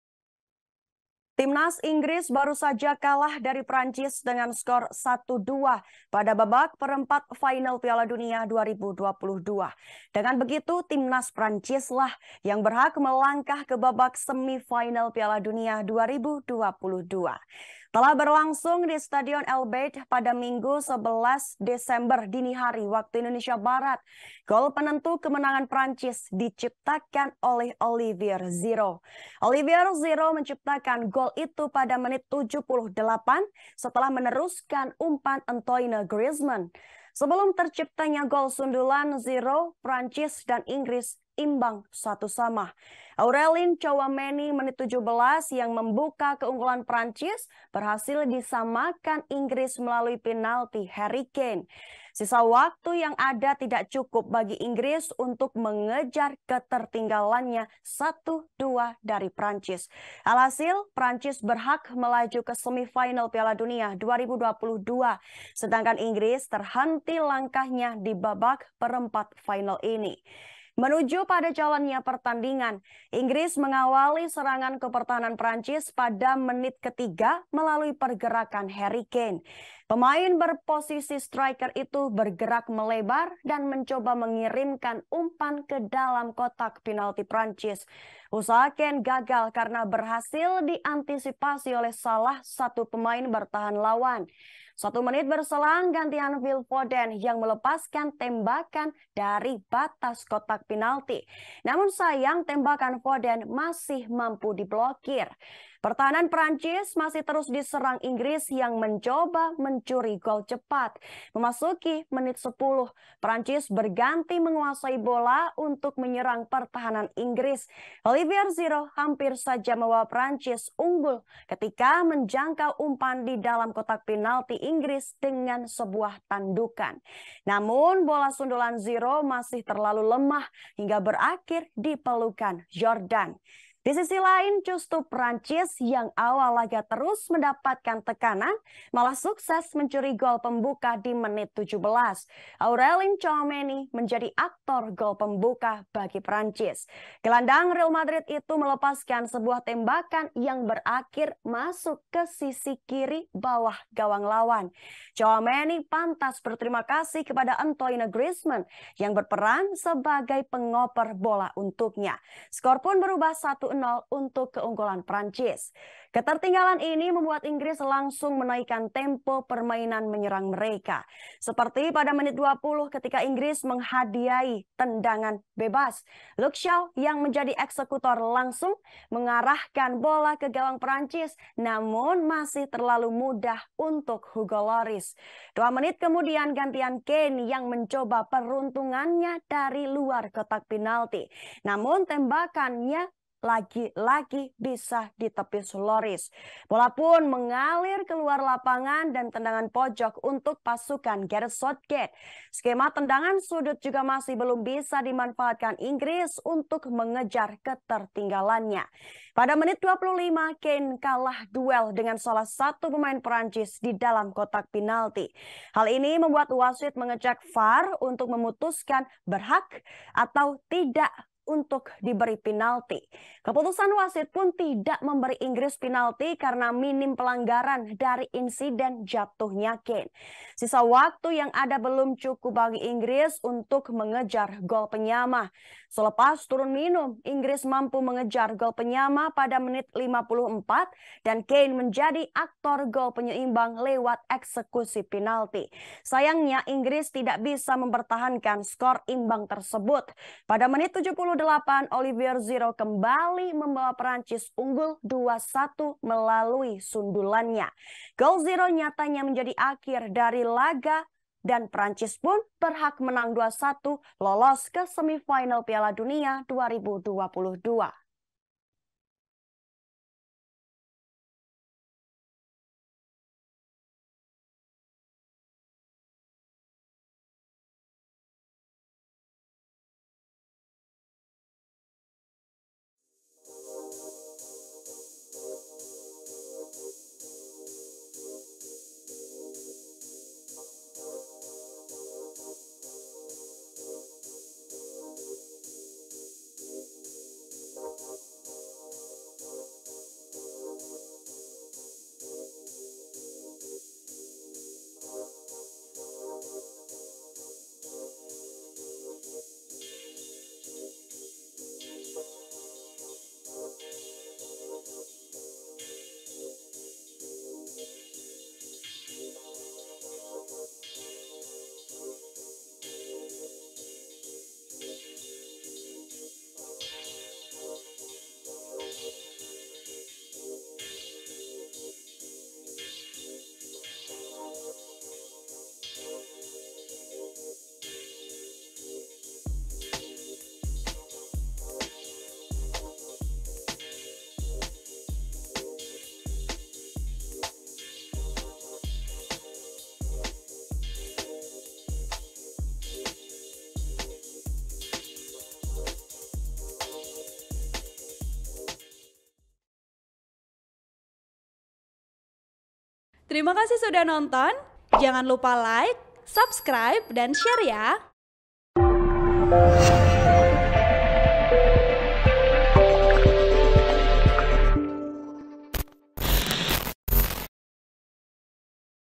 Prancis Lolos Semifinal Piala Dunia 2022 seusai Kalahkan Inggris, Olivier Giroud Cetak Gol Penentu
TRIBUN-VIDEO.COM - Timnas Inggris baru saja kalah dari Prancis dengan skor 1-2 pada babak perempat final Piala Dunia 2022.

Dengan begitu, Timnas Prancis lah yang berhak melangkah ke babak semifinal Piala Dunia 2022.

Telah berlangsung di Stadion Al Bayt, Minggu (11/12) dini hari WIB, gol penentu kemenangan Prancis diciptakan Olivier Giroud.

Olivier Giroud menciptakan gol itu pada menit 78 setelah meneruskan umpan Antoine Griezmann.

Sebelum terciptanya gol sundulan Olivier Giroud, Prancis dan Inggris bermain imbang 1-1.

Aurelien Tchouameni (17') yang membuka keunggulan Prancis berhasil disamakan Inggris lewat penalti Harry Kane (78').

Sisa waktu yang ada, tidak cukup bagi Inggris untuk mengejar ketertinggalannya 1-2 dari Prancis.

Alhasil, Prancis berhak melaju ke Semifinal Piala Dunia 2022 sedangkan Inggris terhenti langkahnya di babak perempat final ini.

Menuju pada jalannya pertandingan, Inggris mengawali serangan ke pertahanan Prancis pada menit ketiga melalui pergerakan Harry Kane.

Pemain berposisi striker itu bergerak melebar dan mencoba mengirimkan umpan ke dalam kotak penalti Prancis.

Usaha Kane gagal karena berhasil diantisipasi oleh salah satu pemain bertahan lawan.

Satu menit berselang, gantian Phil Foden yang melepaskan tembakan dari batas kotak penalti, amun sayang, tembakan Foden masih mampu diblokir.

Pertahanan Prancis masih terus diserang Inggris yang mencoba mencuri gol cepat.

Memasuki menit 10, Prancis berganti menguasai bola untuk menyerang pertahanan Inggris yang dikomandoi John Stones dkk.

Olivier Giroud hampir saja membawa Prancis unggul ketika menjangkau umpan di dalam kotak penalti Inggris dengan sebuah tandukan.

Namun bola sundulan Giroud masih terlalu lemah, hingga berakhir dipelukan Jordan Pickford.

Di sisi lain, justru Prancis yang awal laga terus mendapatkan tekanan malah sukses mencuri gol pembuka pada menit 17.

Aurelien Tchouameni menjadi aktor gol pembuka Prancis.

Gelandang Real Madrid itu melepaskan sebuah tembakan yang berakhir masuk ke sisi kiri bawah gawang lawan.

Tchouameni pantas berterima kasih kepada Antoine Griezmann yang berperan sebagai pengoper bola untuknya.

Skor pun berubah 1-0 untuk keunggulan Prancis.

Ketertinggalan ini membuat Inggris langsung menaikkan tempo permainan menyerang mereka.

Seperti pada menit 20 ketika Inggris dihadiahi tendangan bebas.

Luke Shaw yang menjadi eksekutor langsung mengarahkan bola ke gawang Prancis, namun masih terlalu mudah untuk Hugo Lloris.

Dua menit kemudian gantian Kane yang mencoba peruntungannya dari luar kotak penalti, namun tembakan yang lagi-lagi masih bisa ditepis Lloris.

Bola pun mengalir ke luar lapangan dan tendangan pojok untuk pasukan Gareth Southgate.

Skema tendangan sudut juga masih belum bisa dimanfaatkan Inggris untuk mengejar ketertinggalannya.

Pada menit 25, Kane kalah duel dengan salah satu pemain Prancis di dalam kotak penalti.

Hal ini membuat wasit mengecek VAR untuk memutuskan berhak diberi penalti atau tidak.

Keputusan wasit pun tidak memberi Inggris penalti karena minim pelanggaran dari insiden jatuhnya Kane.

Sisa waktu yang ada belum cukup bagi Inggris untuk mengejar gol penyama.

Selepas turun minum, Inggris mampu mengejar gol penyama pada menit 54, dan Kane menjadi aktor gol penyeimbang lewat eksekusi penalti.

Sayangnya Inggris tidak bisa mempertahankan skor imbang tersebut.

Pada menit 78, Olivier Giroud kembali membawa Prancis unggul 2-1 melalui gol sundulannya.

Gol Giroud nyatanya menjadi akhir dari laga, dan Prancis pun berhak menang 2-1 lolos ke Semifinal Piala Dunia 2022.

(Tribun-Video.com)

Artikel ini telah tayang di Tribunnews.com dengan judul Hasil Inggris vs Prancis: Giroud Bawa Les Bleus Segel Tiket Semifinal Piala Dunia 2022,
Penulis: Rochmat Purnomo
Editor: Dwi Setiawan